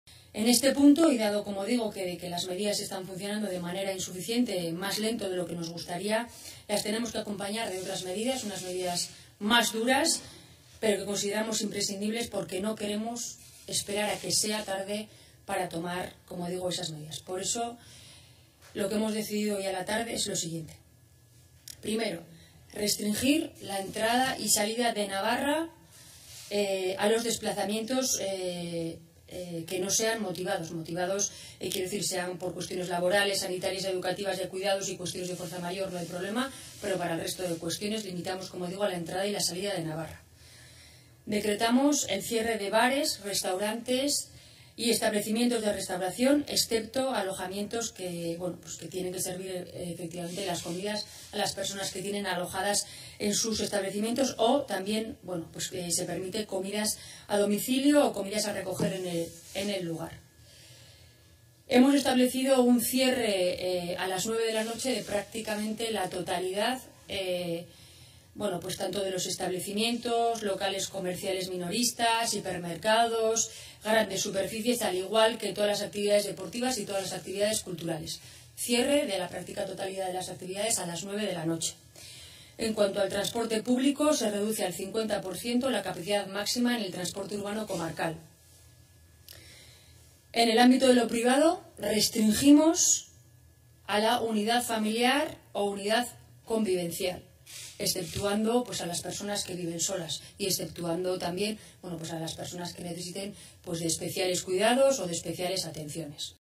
Chivite anuncia el confinamiento perimetral de Navarra
El Gobierno de Navarra ha decidido restringir la entrada y salida de la Comunidad foral a los desplazamientos que no sean motivados y también cerrará los bares y restaurantes a partir de este jueves 22 de octubre hasta el 4 de noviembre.   Así lo ha anunciado la presidenta del Gobierno foral, María Chivite.

(Fuente: Gobierno de Navarra)